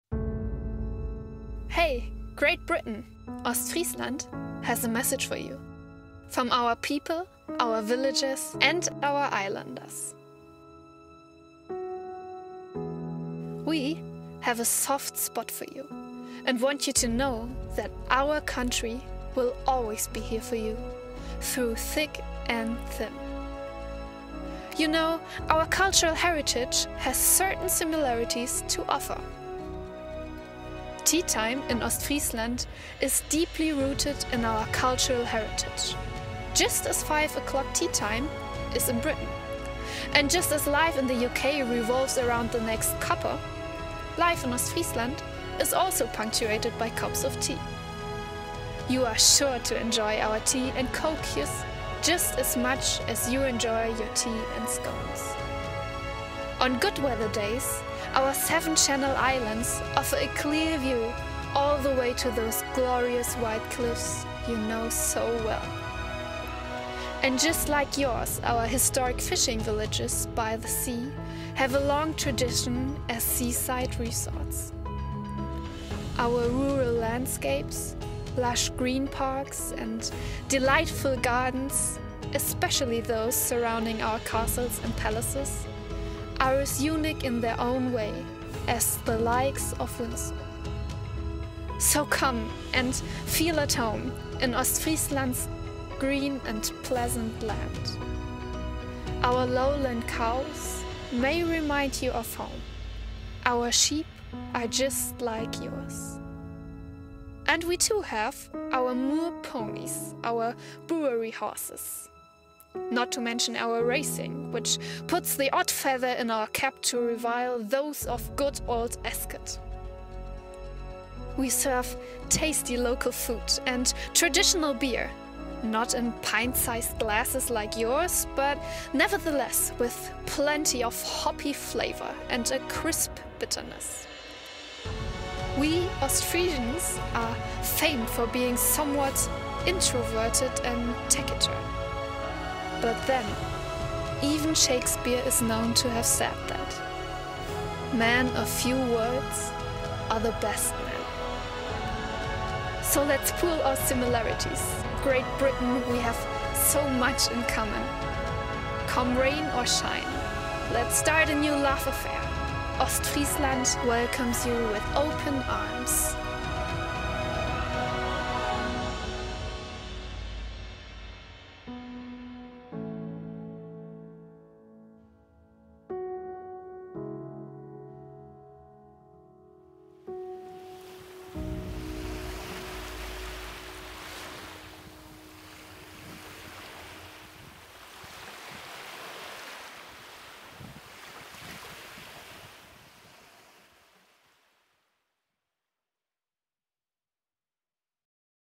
Ostfriesland: Message to Great Britain
Dear Great Britain,
come rain or shine - our country will always be here for you.
We have so much in common - let's keep our friendship.

Hey Great Britain,
Ostfriesland has a message for you,
from our people, our villagers and our islanders.
We have a soft spot for you, and want you to know
that our country will always be here for you,
Because what do you know? Our cultural heritage has certain similarities to offer.
Teatime in Ostfriesland
is deeply rooted
in our cultural heritage, just as five o’clock teatime is in Britain.
And just as life in the UK revolves around the next cuppa,
life in Ostfriesland is also punctuated by cups of tea.

You are sure to enjoy our tea and Kookjes
just as much as you enjoy your tea and scones.
On good weather days,
our seven channel islands
offer a clear view
all the way to those glorious white cliffs you know so well.
And just like yours, our historic fishing villages
by the sea,
have a long tradition as seaside resorts.
Our rural landscapes,
lush green parks,
and delightful gardens ,
especially those surrounding our castles
and palaces,
are as unique in their own way as the likes of Windsor –
so come and feel at home - in Ostfriesland’s green and pleasant land.
Our lowland cows
may remind you of home;
our sheep
are just like yours.
And we too have our moor ponies, our brewery horses,
and our racing -
not to mention the odd feather in our cap to rival those
of good old Ascot.
We serve tasty local food
and traditional beer,
not in pint-sized glasses like yours -
but nevertheless with plenty of ‘hoppy flavour’ and a ‘crisp bitterness’.
We Ostfriesians are famed for being somewhat introverted and taciturn -
but then even Shakespeare is known to have said that:
‘Men of few words are the best men.’
So let’s pool our similarities.
Great Britain, we have so much in common.
Come rain or shine, let’s start a new love affair.
Ostfriesland welcomes you with open arms.

Actor/Voice: Sylvie Gühmann, Writer of "Fettnäpfchenführer Ostfriesland" and "Die junge Frau und das Meer", Conbook Verlag